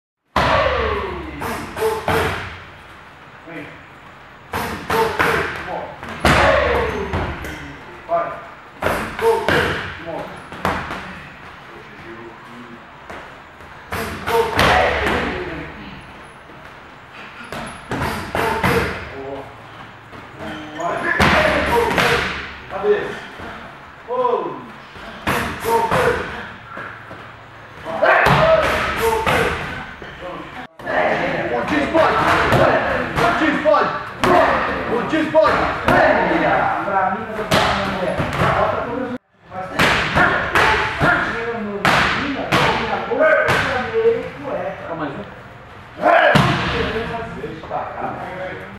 MASSARANDUBA TREINANDO CHUTES E JOELHADAS
Vídeo exclusivo do blog MMA Por Dentro da Arena . Francisco Massaranduba treinando chutes e joelhadas em preparação para a luta contra Gleison Tibau no UFC Rio 3.